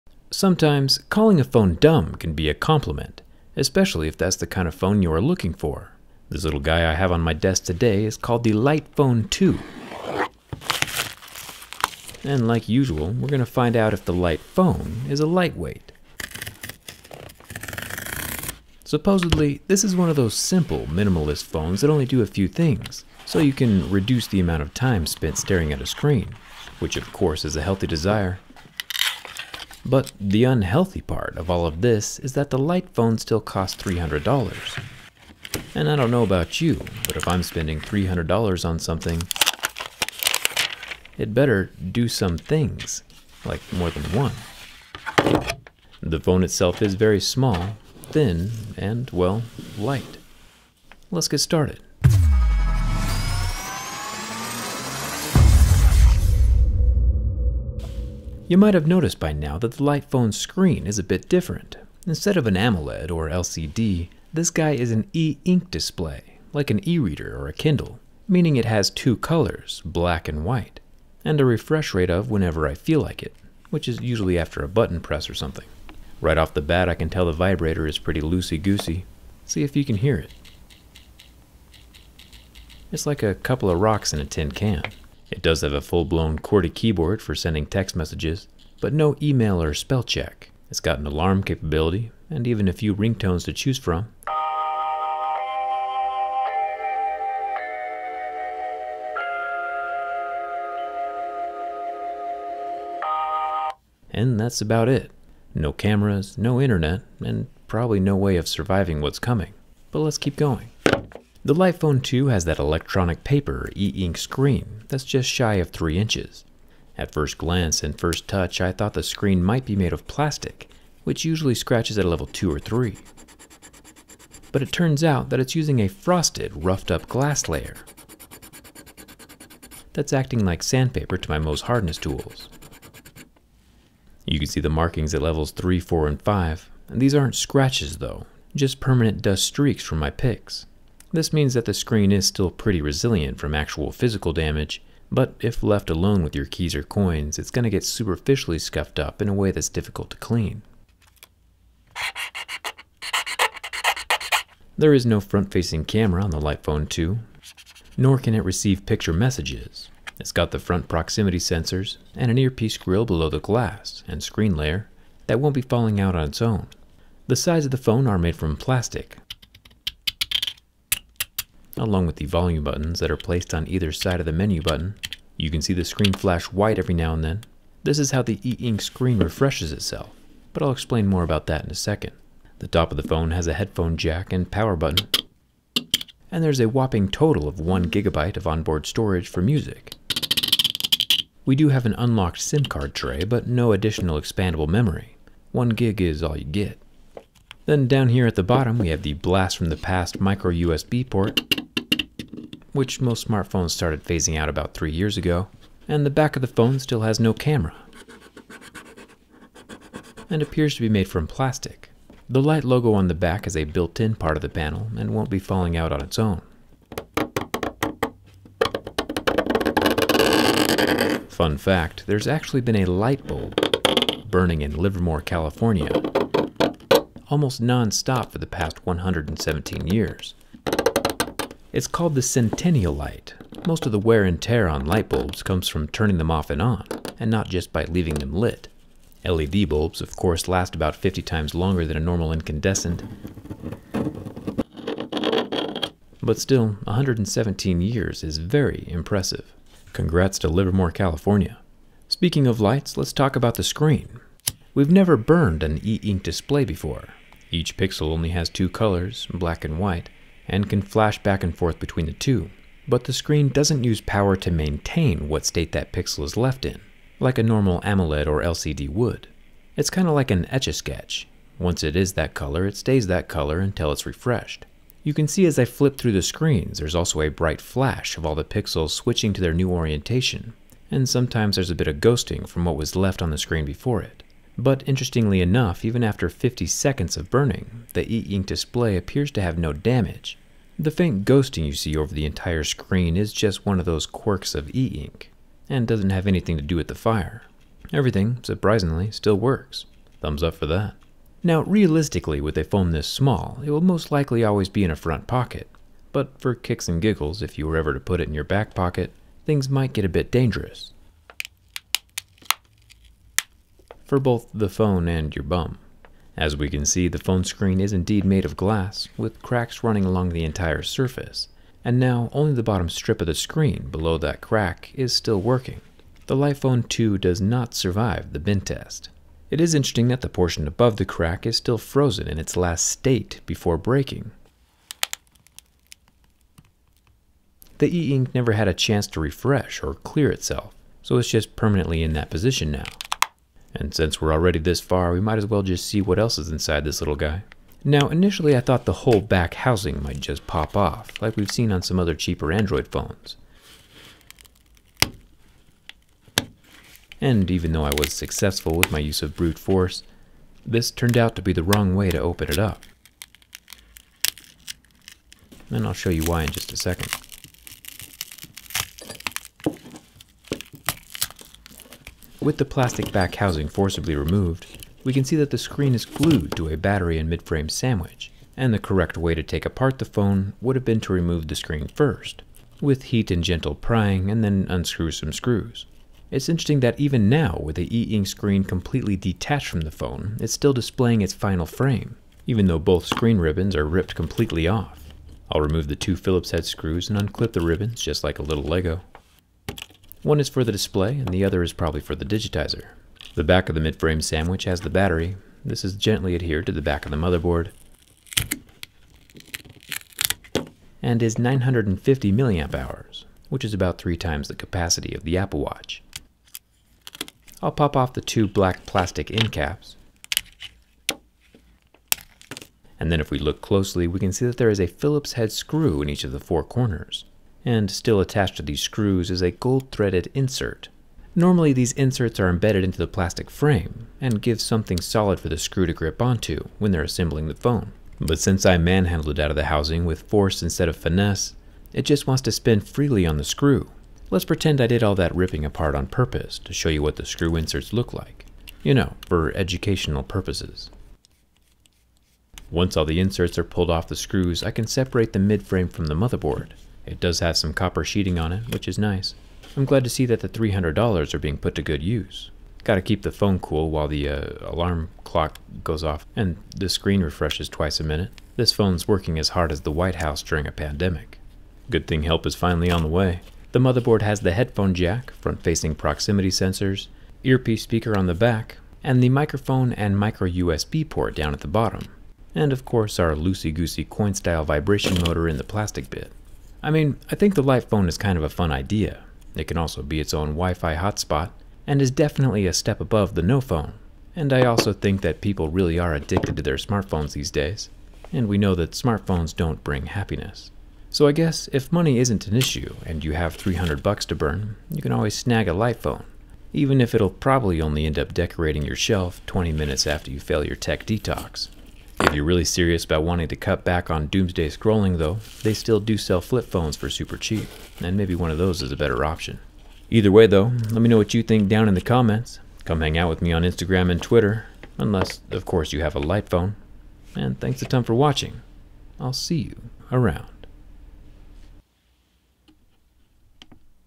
The Dumbest Smartphone?! - Durability Test!
Sometimes people yearn for the simple life. And that simple life... can be expensive. This 'Light Phone 2' is a very expensive $300 minimalist smartphone. The Light Phone 2 is dumbed down to just handle the basics. Texting, calling, but can still be a hotspot for a laptop or 2nd device if needed. Making this the dumbest smartphone weve tested so far. Or is it the smartest dumb phone... who knows. You can check out the light phone yourself This video is obviously not sponsored.

I wont be selling it officially for a few more days, but here is a sneak peak